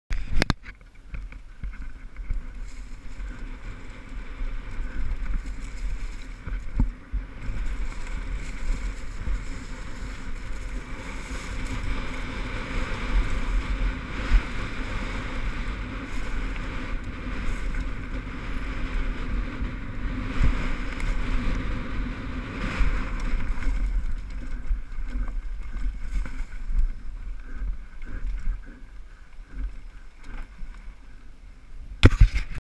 Spaarky - Zlin malenovice air trail 3
Muj nejoblibenejsi trail ze zlina 2 pekne polety po sobe :-)